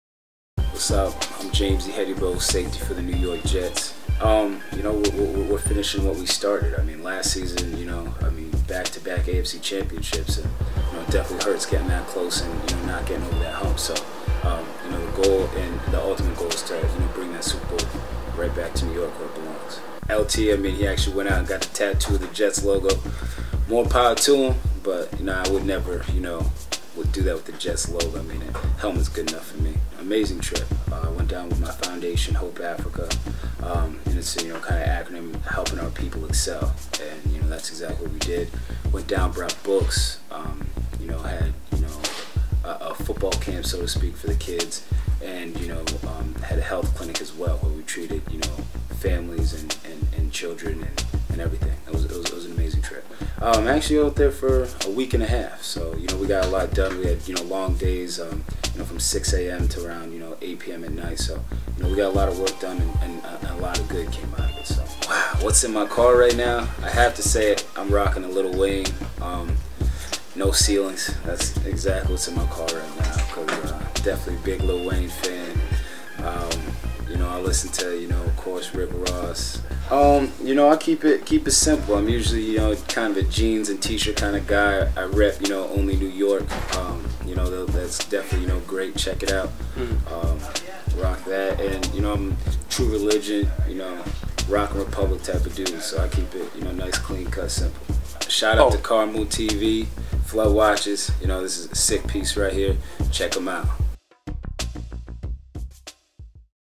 KarmaloopTV x James Ihedigbo
KarmaloopTV talks with NY Jets safety James Ihedigbo.